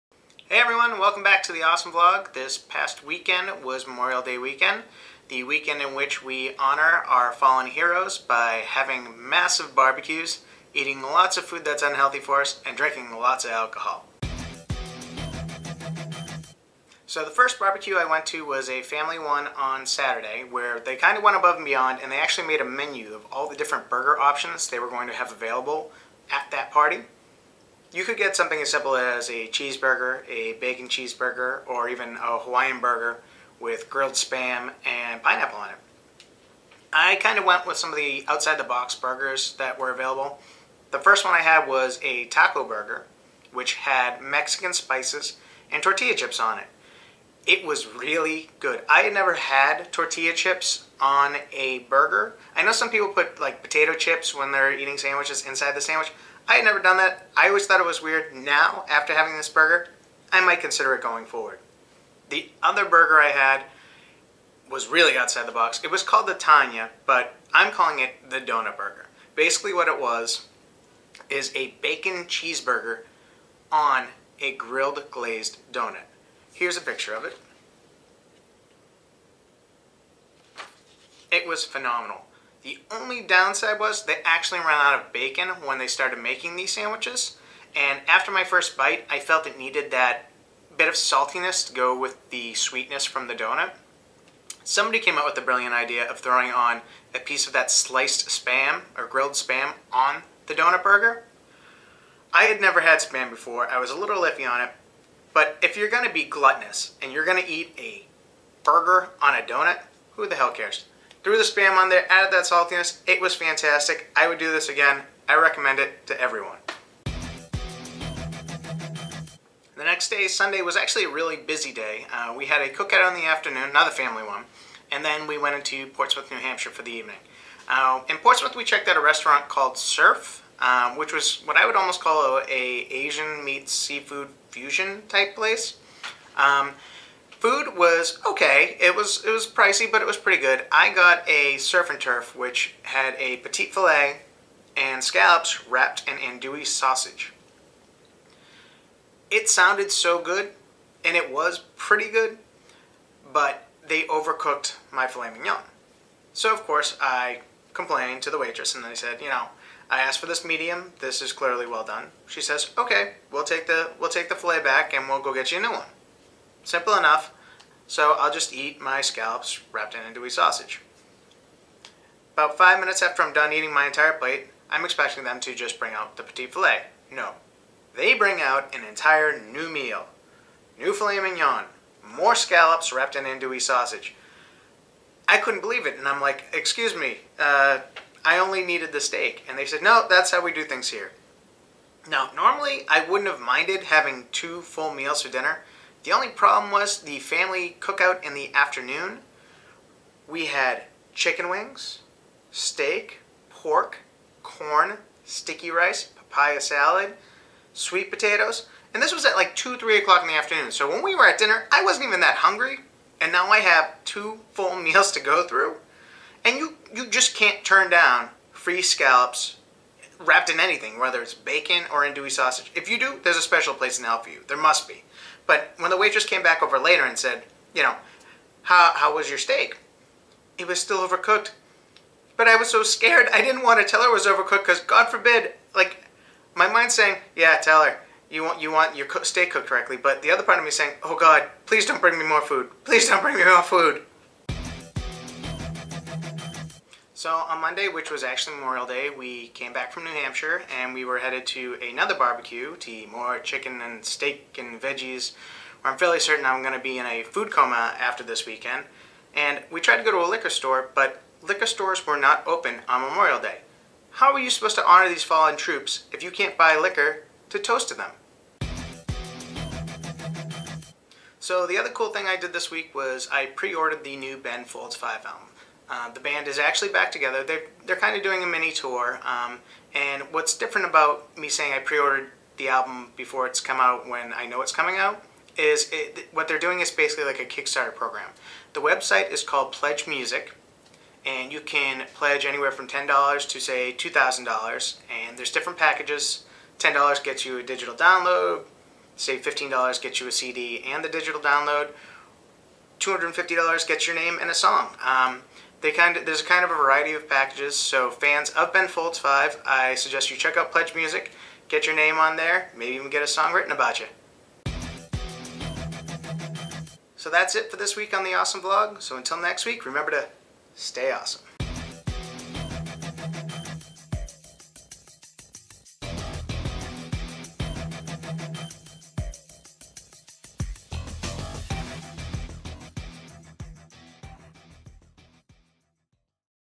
The Awesome Vlog! - Ben Folds Burgers
A recap of Memorial Day weekend and Ben Folds Five reunite.